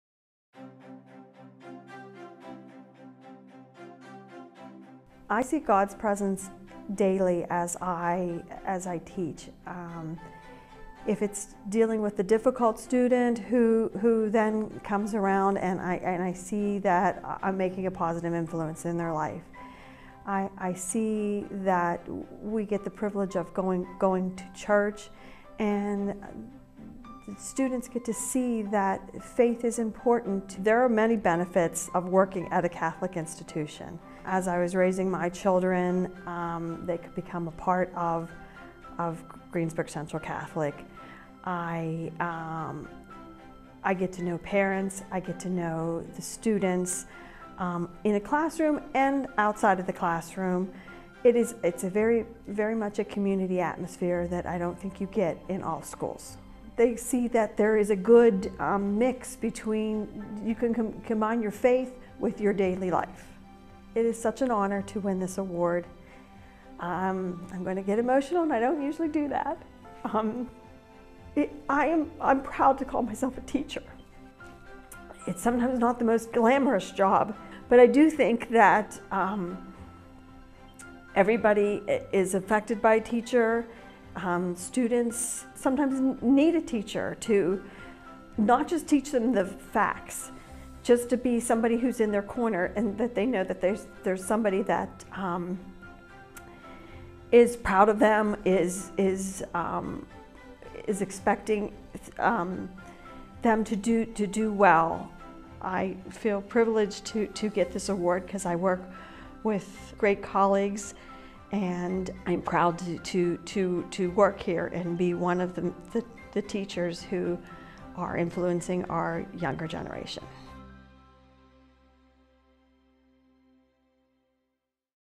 Golden Apple Award winner Colleen Kinkead
Colleen Kinkead, a math teacher at Greensburg Central Catholic Junior-Senior High School, talks about teaching in a Catholic school and winning the Golden Apple Award for excellence in teaching.